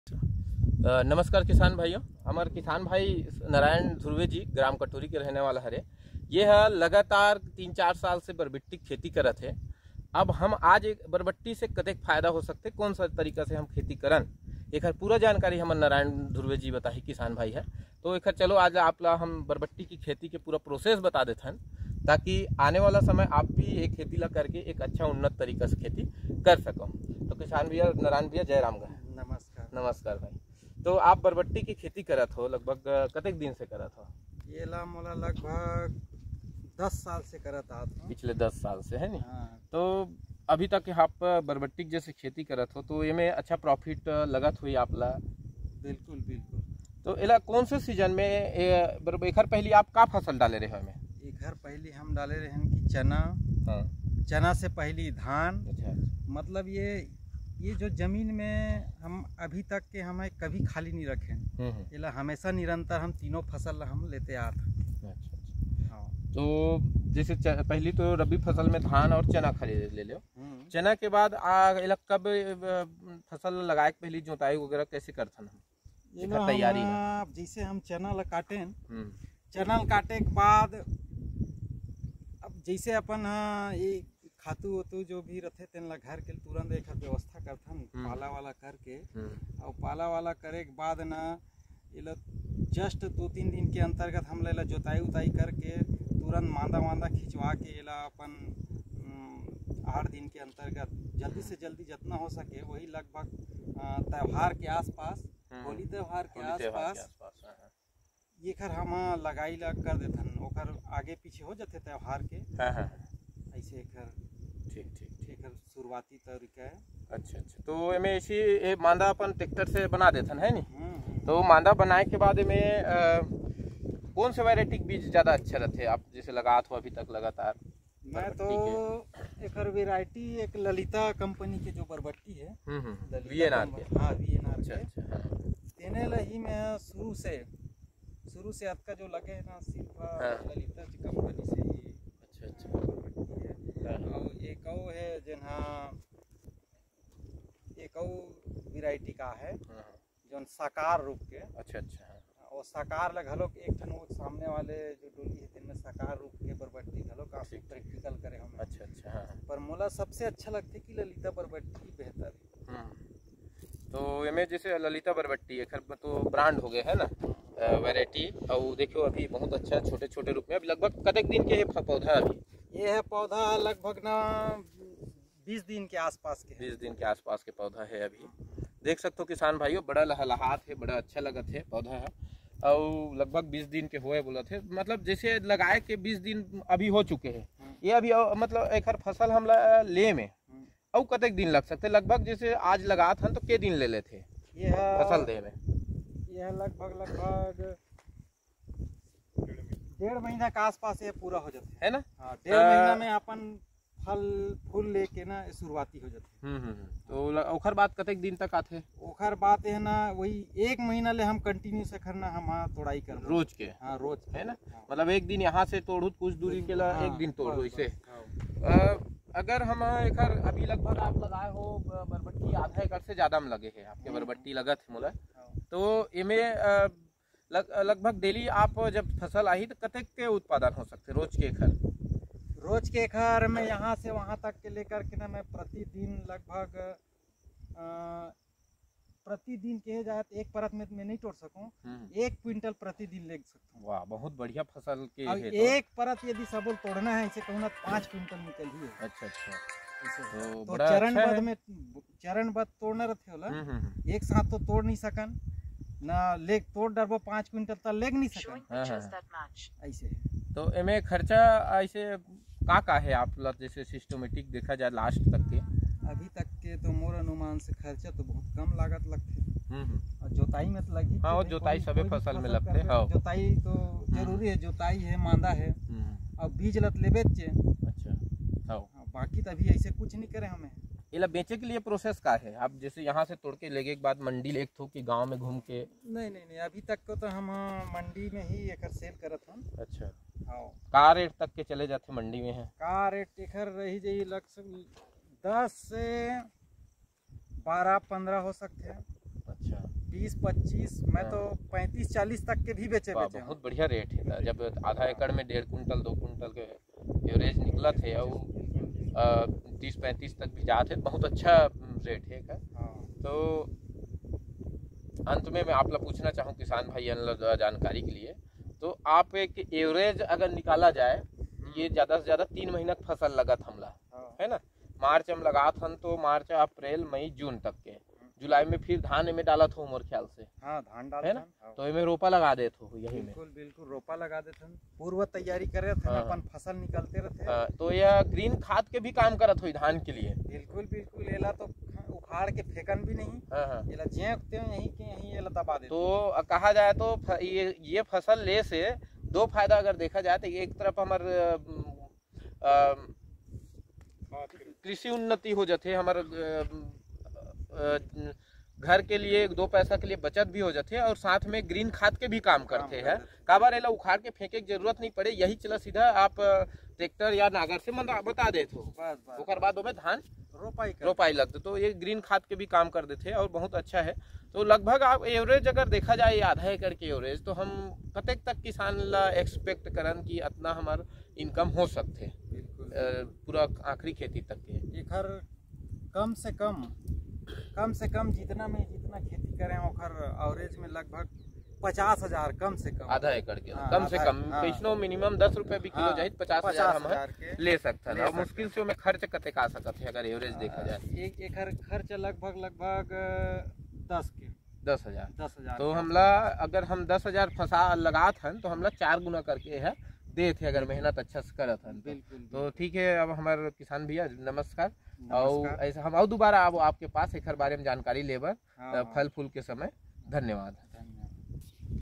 बरबटी की खेती कैसे करें | How to cultivate barbati farming | गर्मी में बरबटी की खेती
ग्राम कटोरी के किसान श्री नारायण ध्रुवे जी  से बरबटी के खेती के बारे में चर्चा हूवा उन्होंने बताया कि चना काटने के बाद खाली जमीन पर मार्च से जून जुलाई तक बरबटी की फसल लगा कर बहुत ही अच्छा कमाई कर रहा है